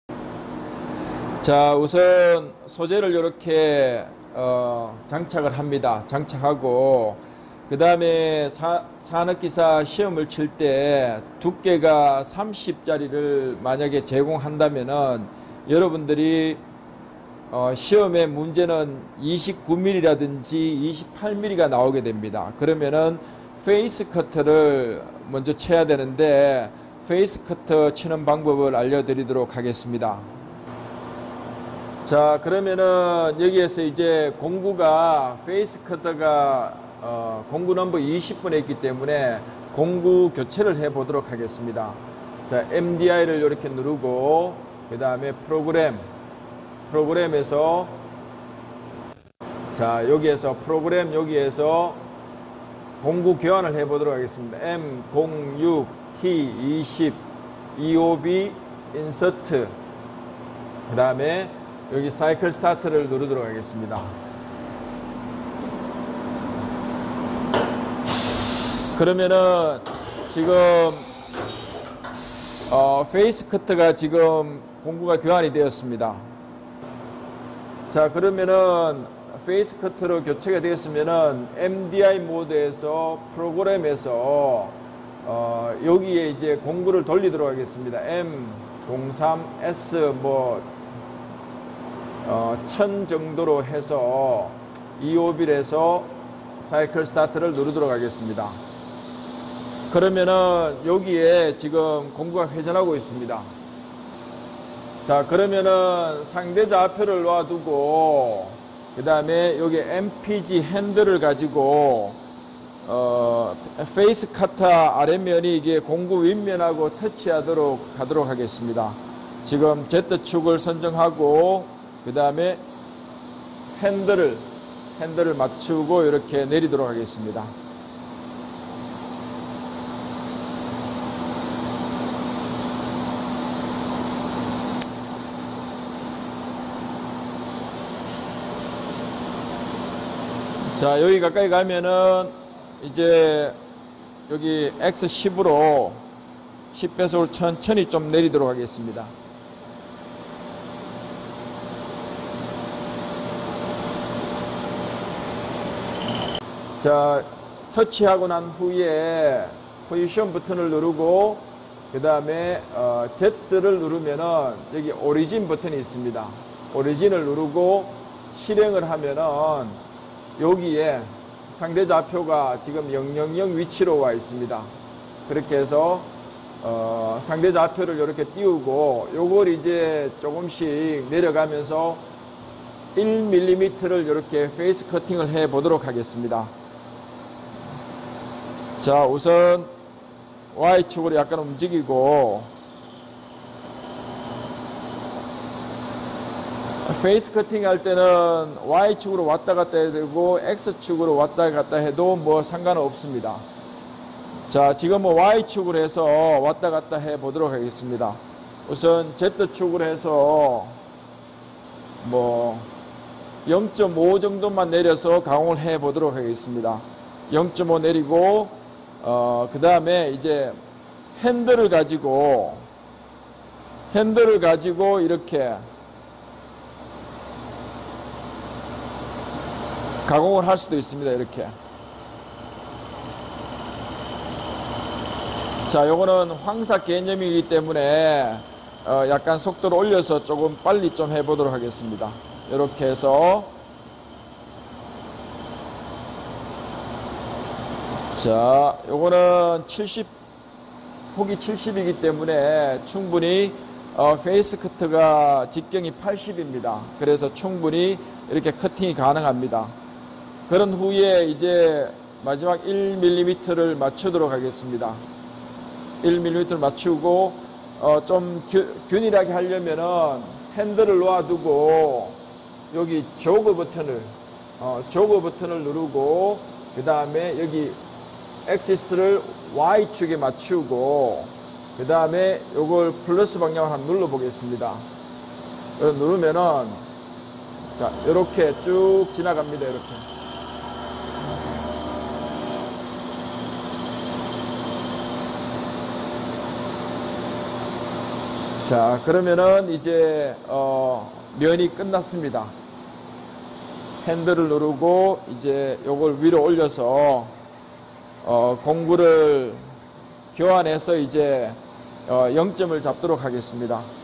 머시닝센터(2) : FACE CUTTING
가공자격증 대비 FACE CUTTING 공정 소개